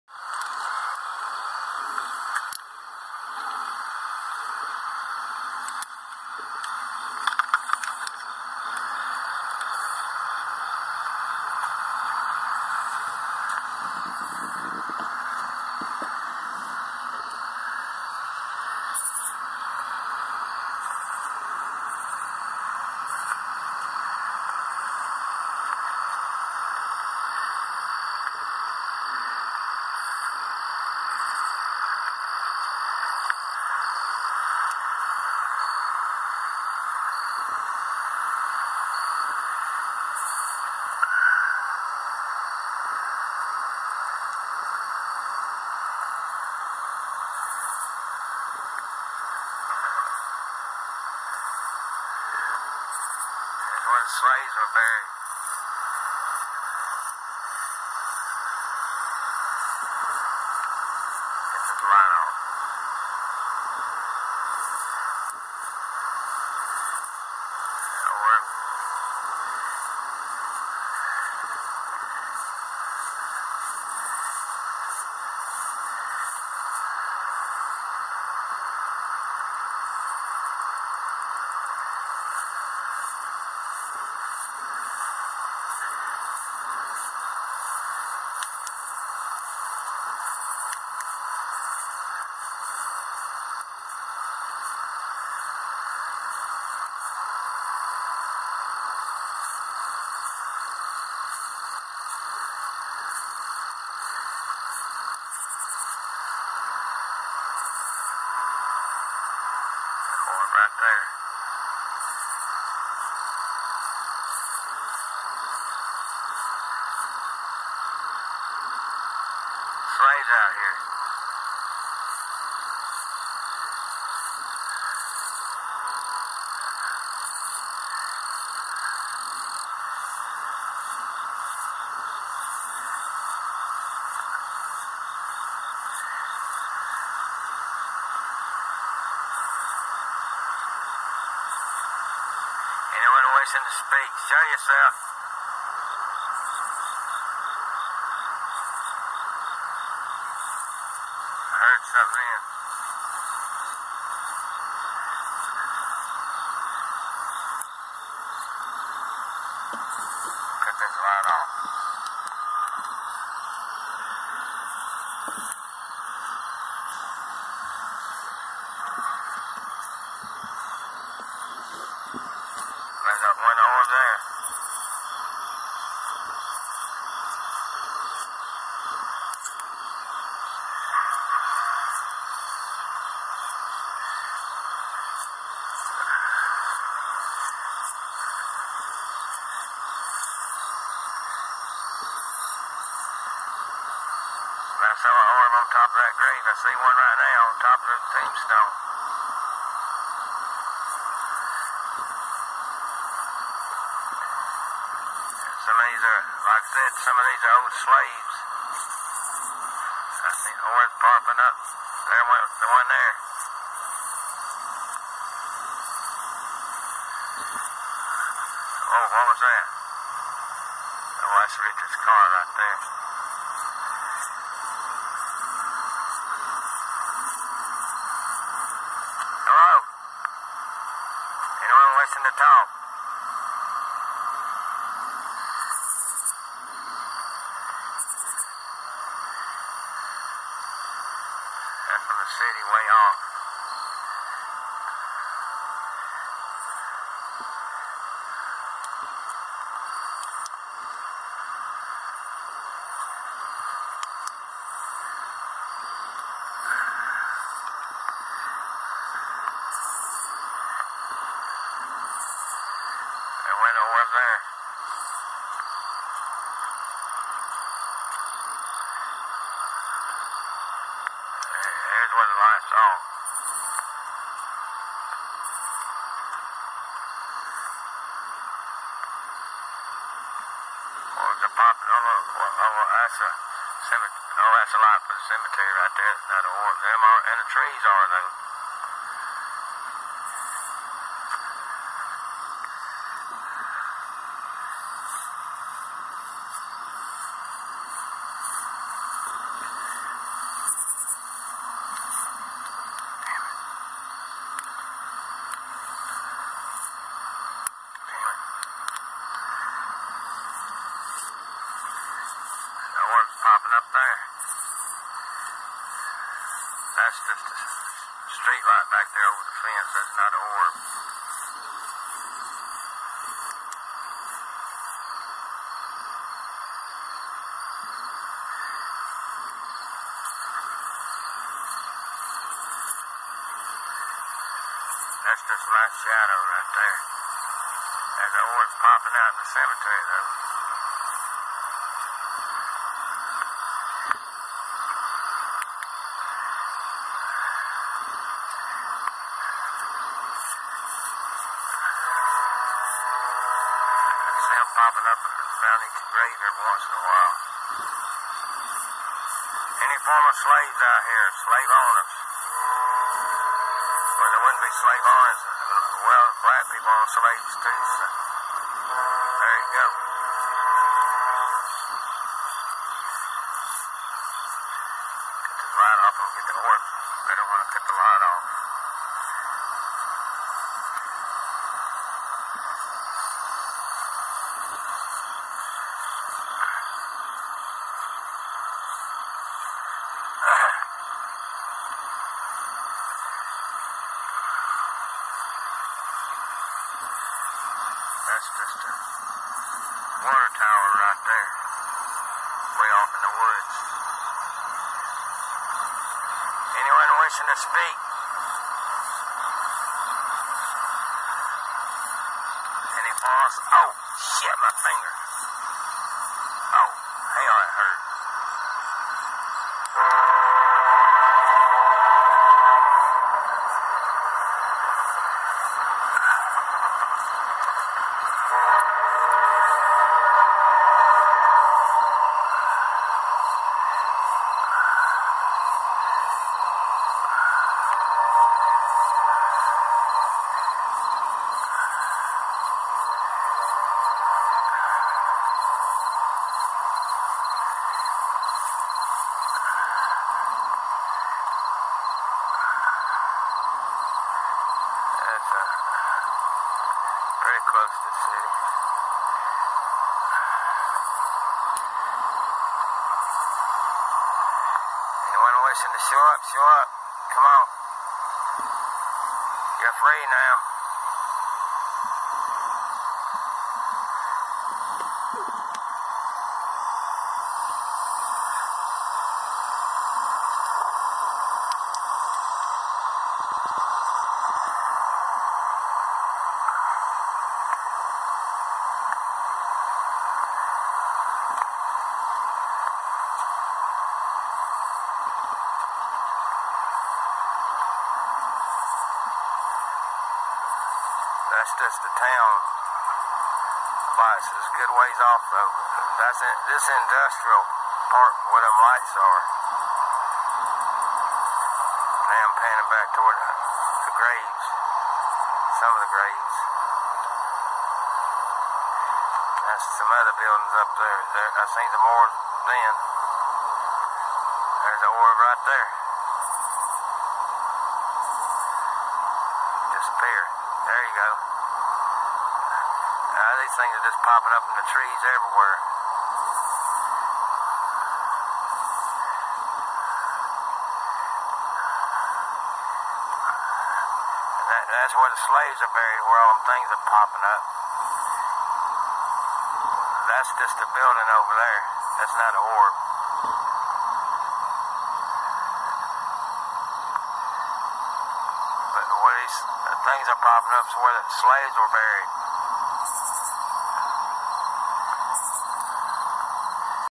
Slave ghost orbs at Rosehill Cemetery Manassas Virginia lots of orbs and City Lights also!
Both ghost orbs and city lights but real orbs and sounds in slave Cemetery!